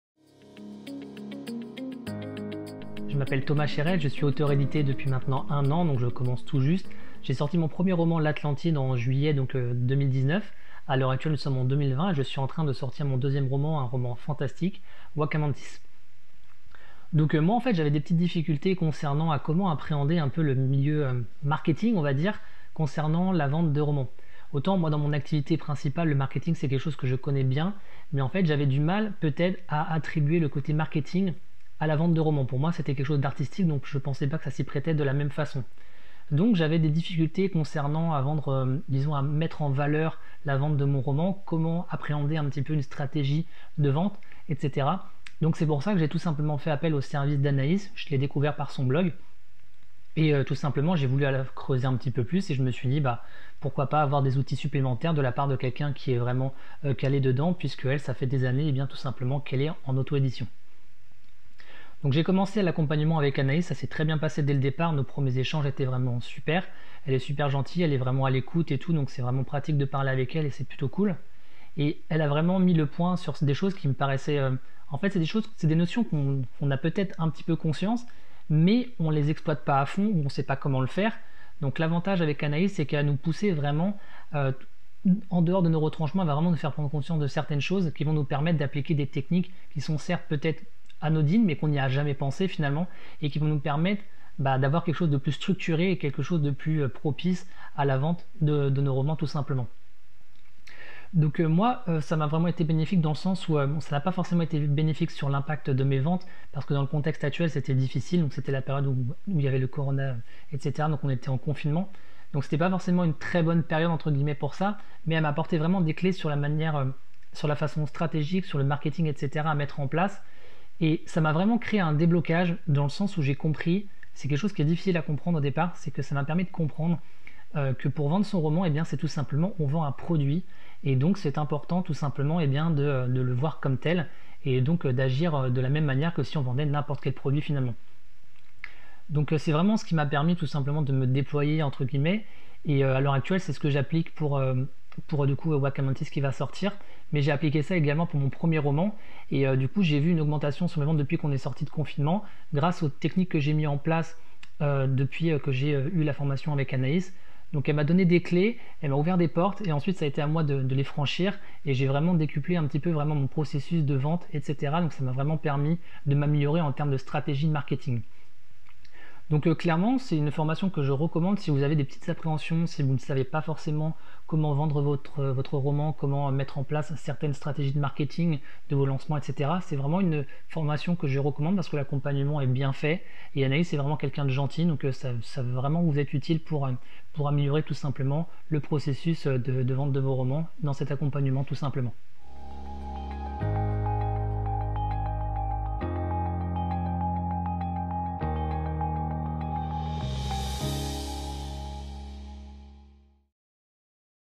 Temoignage de Thomas Chérel - coaching en auto-édition avec Anaïs Weibel
Au début de l'année 2020, j'ai eu l'immense plaisir d'accompagner Thomas Chérel dans ses débuts en auto-édition.

Ensemble, nous avons repris les bases de son travail d'auteur : comment communiquer sur son livre ? Comment le mettre en valeur et le vendre ?

Toutes ces questions et bien plus encore ont été abordées lors de cet accompagnement pour préparer la sortie de son deuxième roman « Wakamentis" !

Thomas a notamment pu voir une augmentation des ventes de son premier roman grâce à l’application de mes différents conseils. :D

✅ Besoin d'aide pour publier votre livre en auto-édition sereinement et en mettant en place toutes les bases pour construire votre lectorat sur le long terme ?